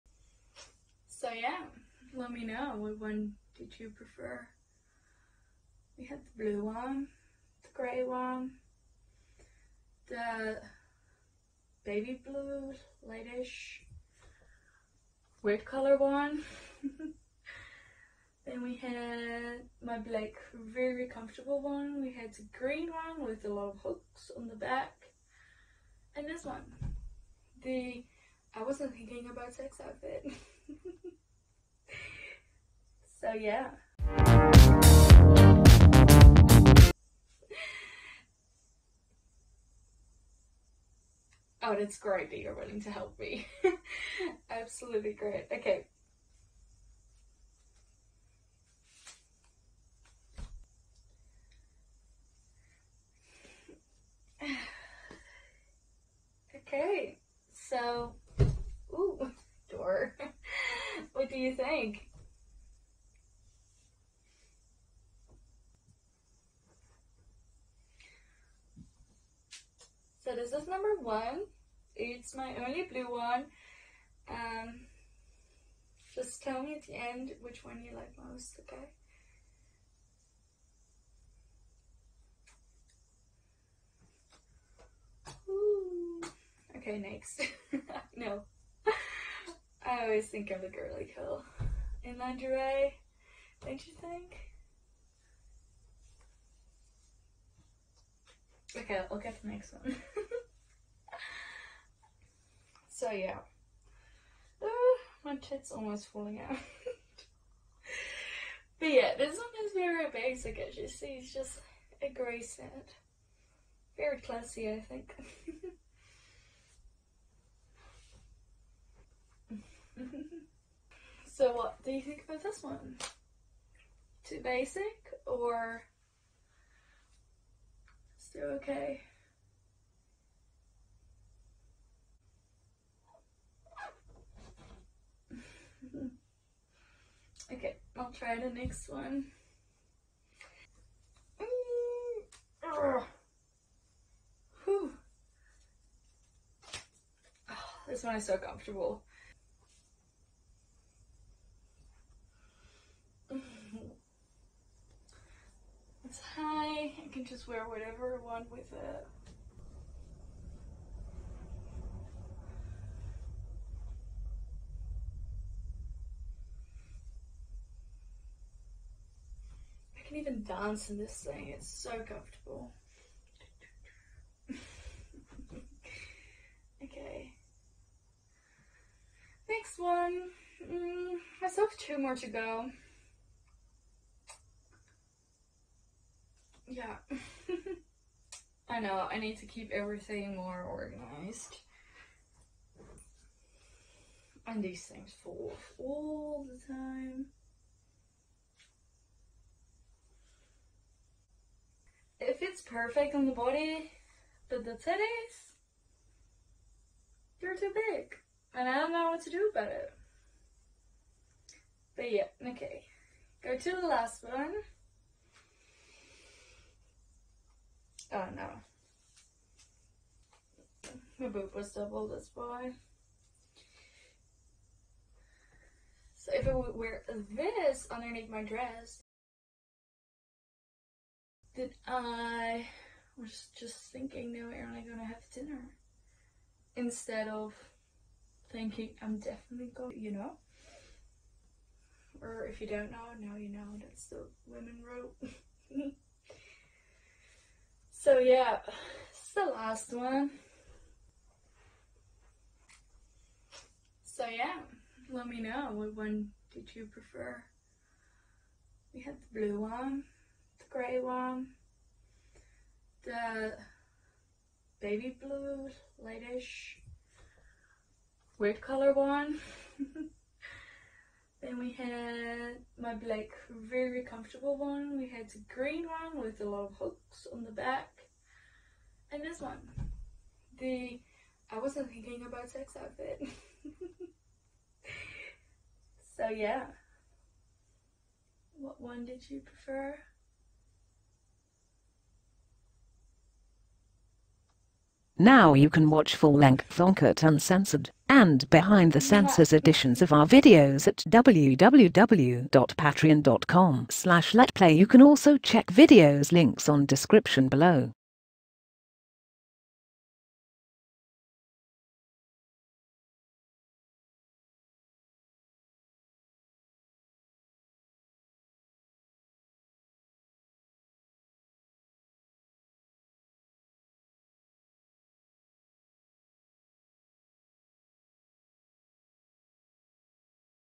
PLUS SIZE LINGERIE TRY ON HAUL
In this video, the beautiful Jordy is trying New Plus Size Lingerie of 2020 .
Let look at New Plus Size Lingerie of 2020. Find New Plus Size Lingerie of review and demonstration by try on, help you for selection of right New Plus Size Lingerie of. Perfect lingerie perfect Model.